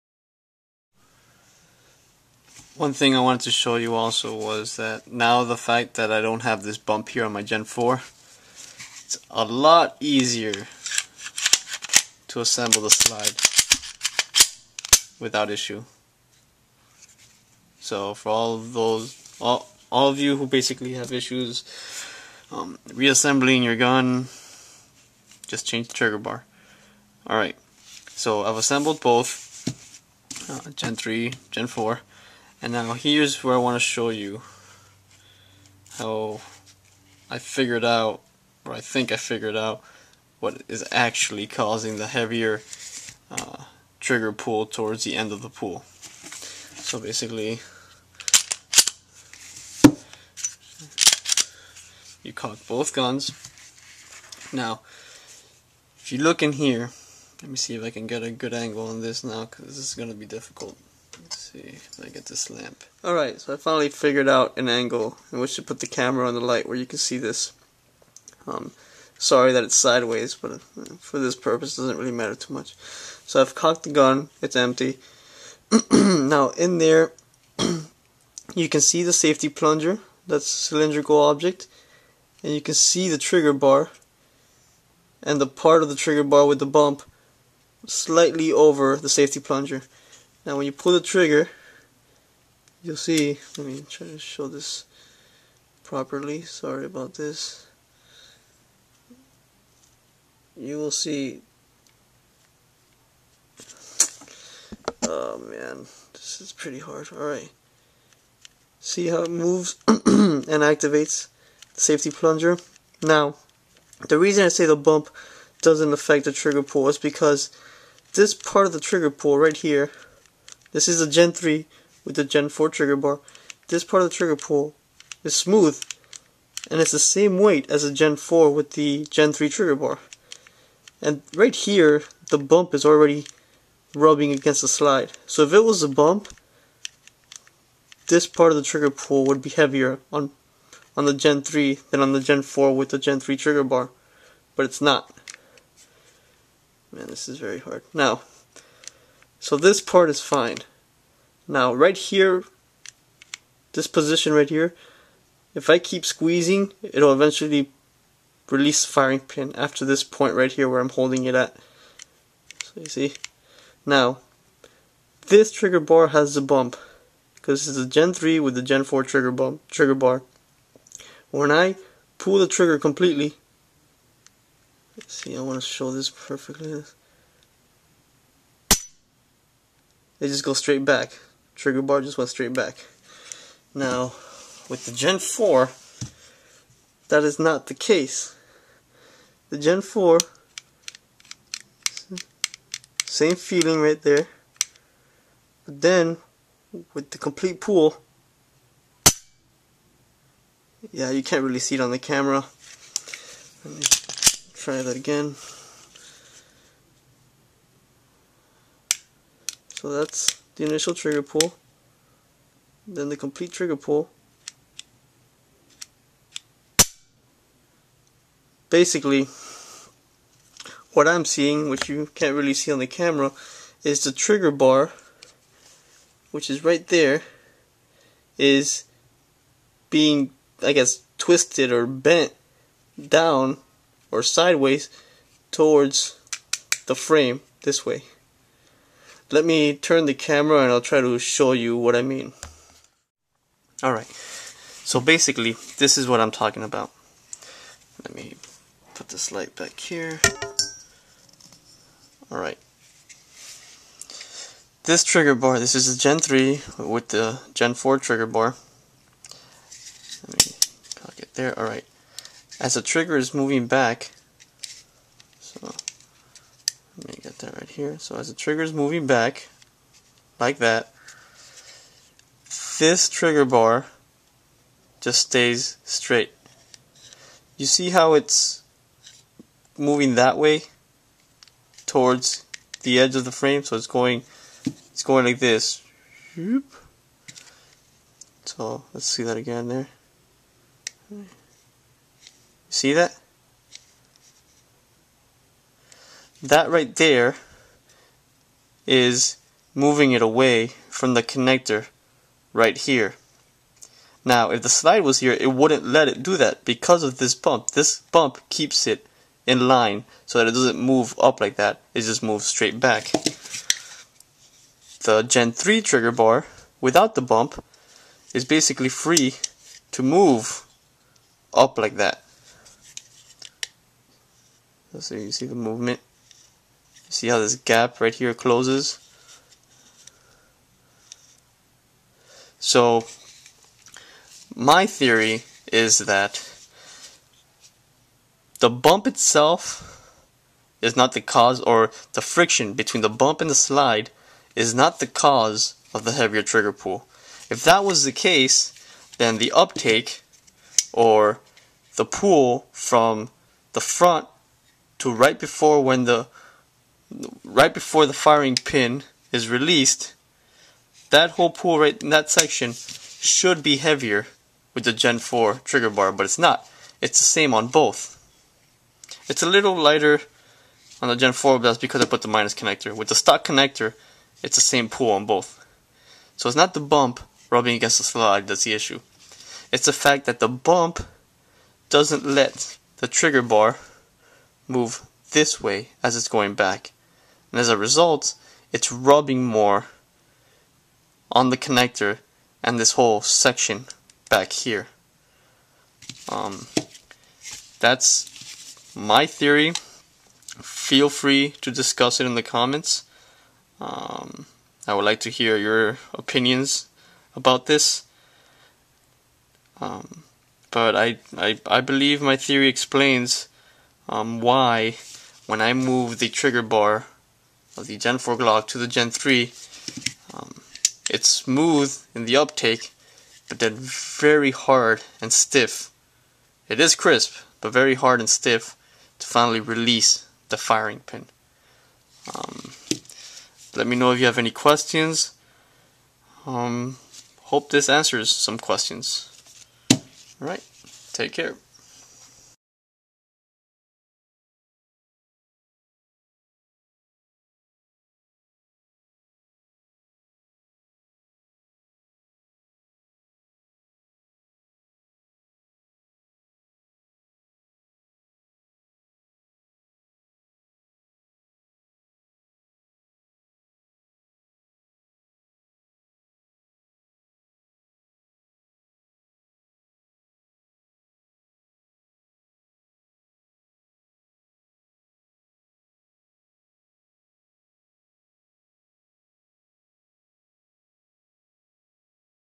What Causes the Glock Gen4 Trigger Weight - Part2
This video shows why the Gen 4 Glocks have a heavier trigger pull compared to the Gen 3. I talk about the theories that appear on forums and why they are incorrect and then propose my own theory as to why the trigger is heavier and support it with demos.

This is part 2 of a two part series.

My conclusion:
-The heavy up-take of the trigger pull is due to the increase in angle of the connector by 5 degrees in the '+' direction in the Gen4 trigger housing.
-The heavy and crisp break of the trigger is due to the bump on the trigger bar not allowing the trigger bar to shift towards the outer edge of the frame and away from the connector assembly.